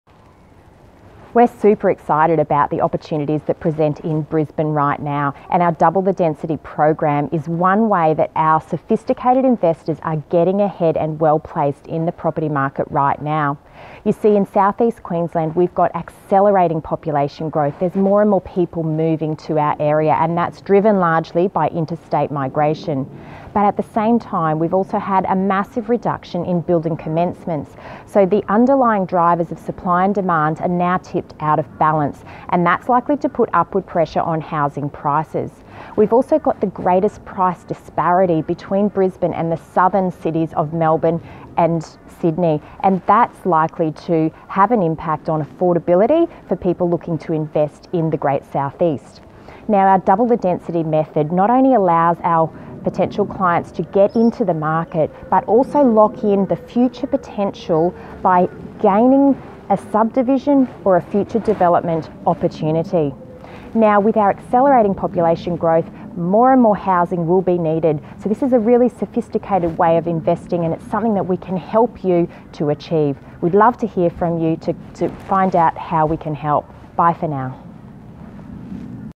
Double the Density Property Investment Strategy - Streamline Property Buyers Brisbane
Right now Brisbane is well placed for future capital growth.  We have accelerating population growth along with declining building commencements.  People need somewhere to live and this is likely to result in upward pressure on housing values.

Using our Double the Density Property Investment technique, now only can uou capitalise on the opportunity today, but you can also lock in the future potential for the future.

Exciting times ahead for Brisbane!